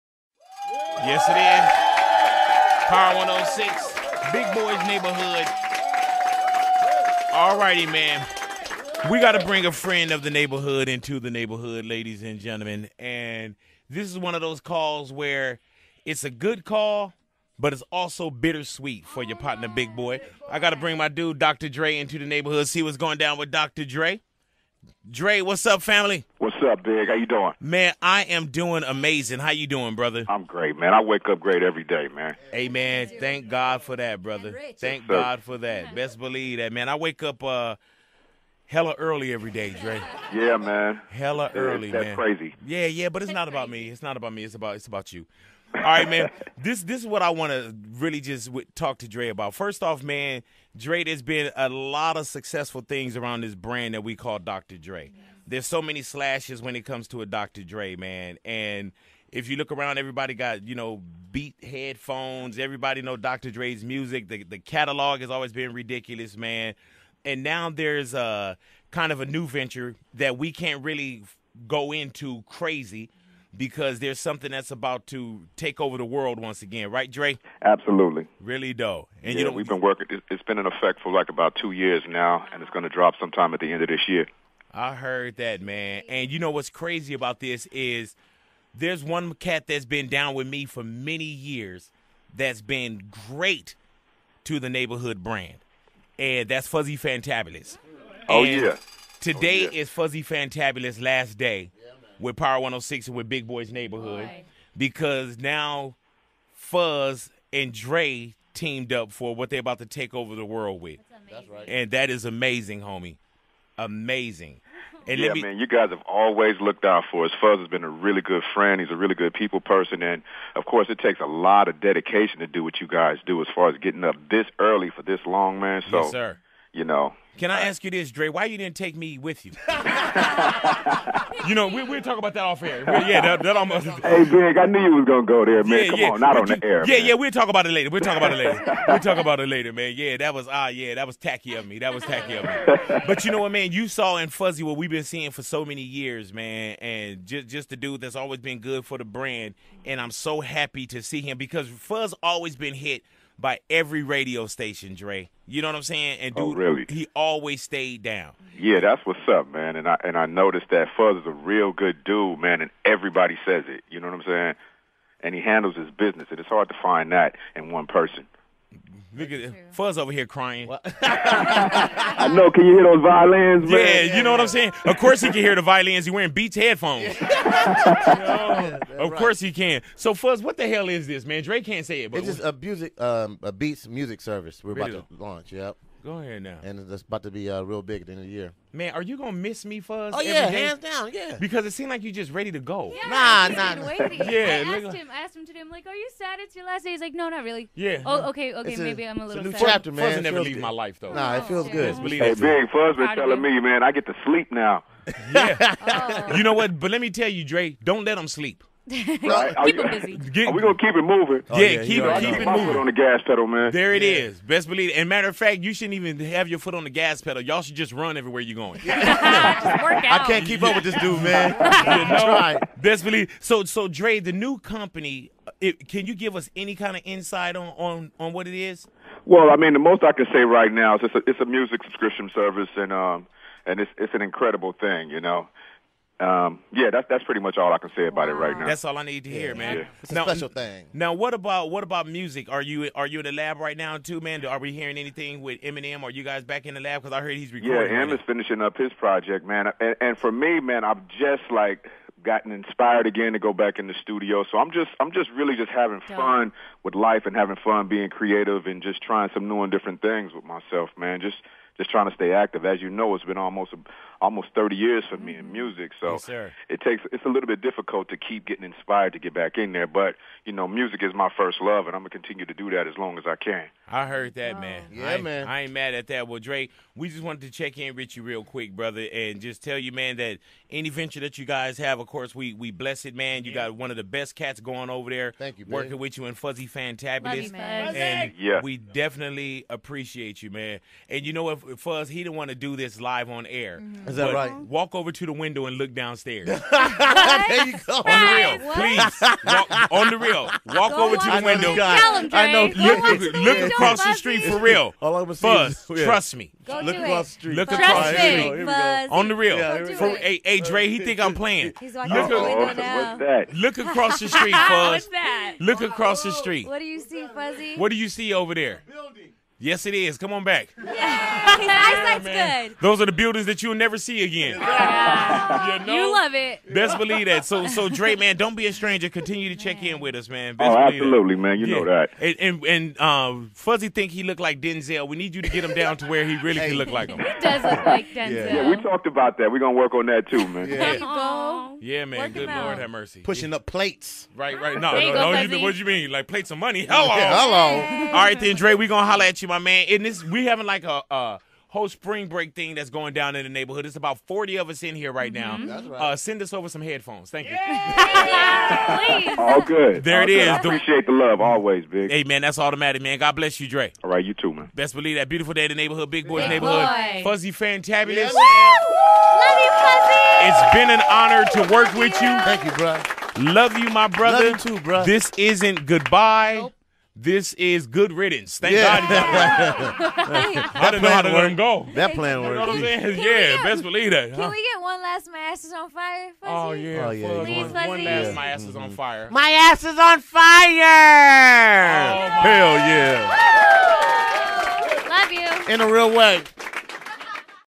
Dr Dre says New Eminem album Almost Done (New Interview 2013)
I can't wait anymore, Can you ??